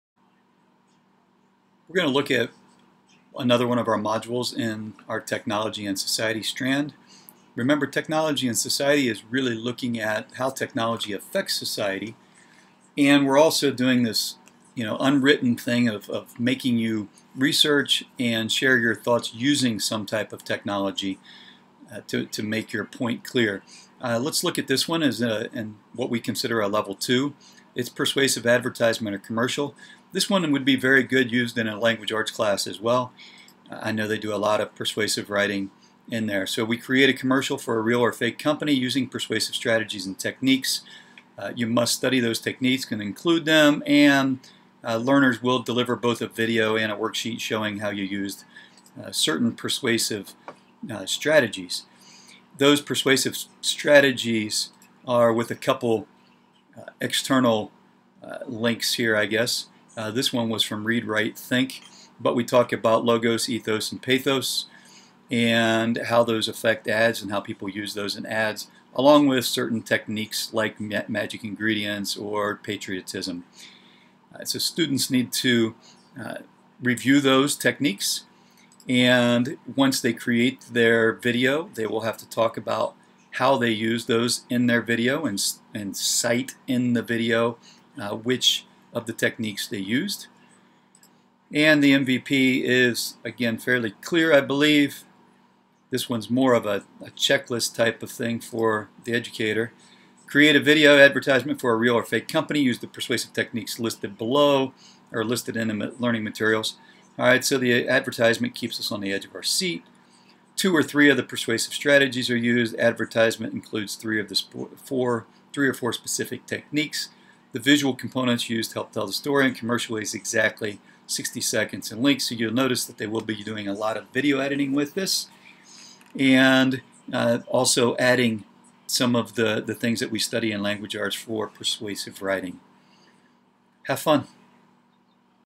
Persuasive Advertisement
This is a short introductory video explaining one of our modules in Middle School Computer Science at New Albany Middle School. This one is about Persuasive Advertising.